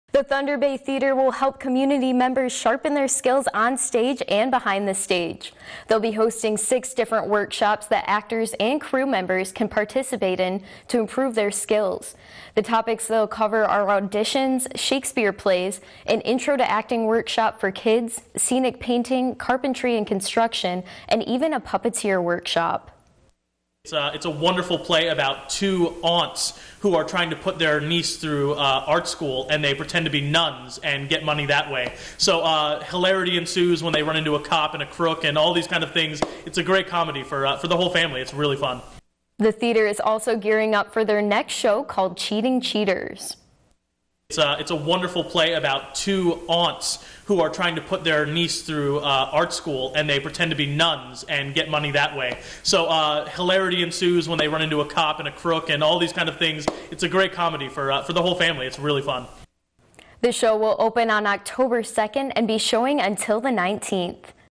Workshops at Thunder Bay Theatre
The Thunder Bay Theatre will help community members sharpen their skills onstage and behind the stage.